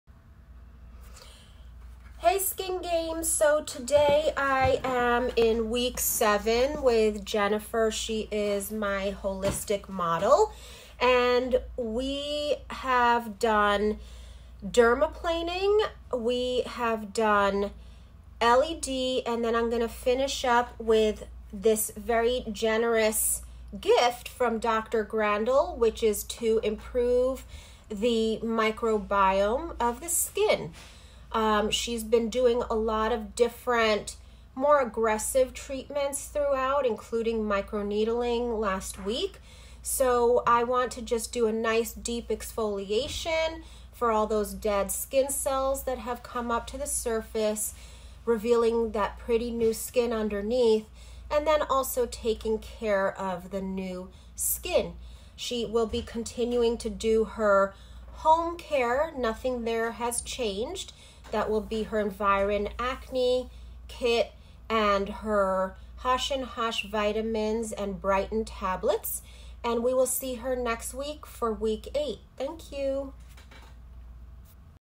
Sabrina Nameri in the Holistic category for TSG2022 - Video 7
Sabrina is making waves with her 7th video for tsg2022 holistic category.
She is one week away from revealing her results!

If you will like to learn more about The Skin Games,